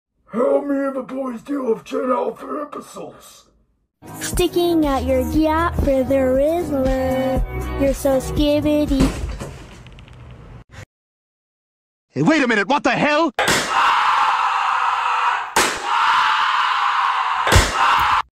How Old Man Larry, Grover and Rey Mysterio deals with Gen alpha
These dudes will make sure no stupid gen alpha bullcrap like gyatt, skibidi, and other slurs will be on this channel, ever.
inspired by @superfeatherdude